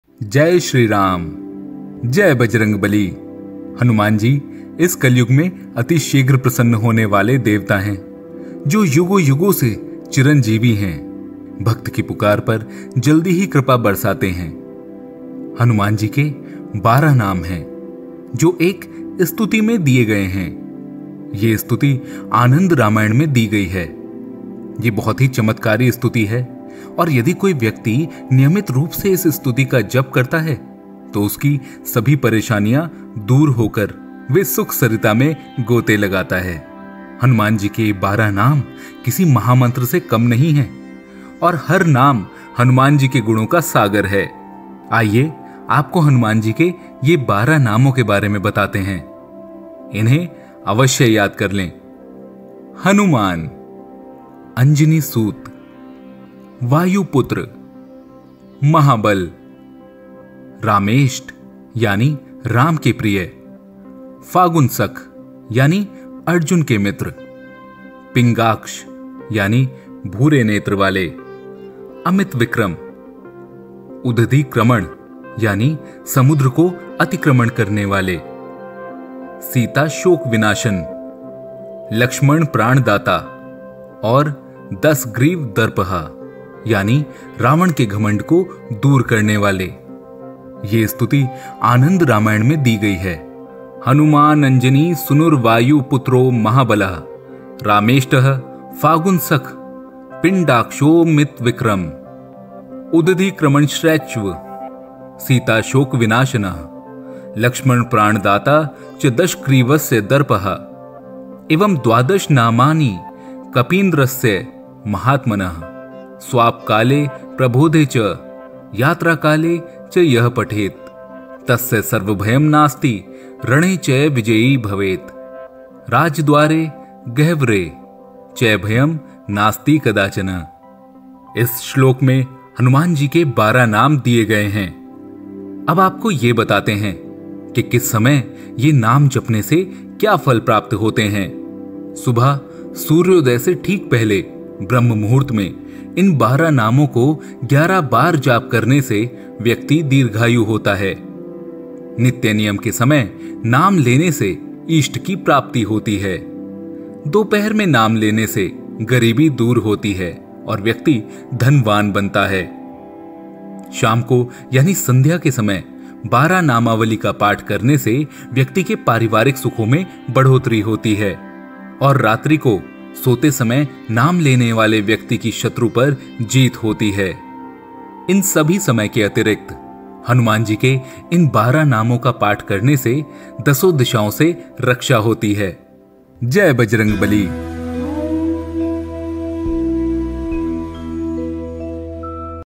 मंगलवार Special : हनुमान जी के ये १२ नाम आज ही रट लें। इनसे हनुमान जी तुरंत प्रसन्न होते हैं।
आज काले या सफेद कपड़े न पहनें।

श्री हनुमत कृपा पाएं 😊

मंगल को भौम भी कहते हैं।

Wear Red & Yellow clothes on Tuesday.

Rituals to follow every day for worshipping Hanuman ji.

Follow us @TheAstrologic.com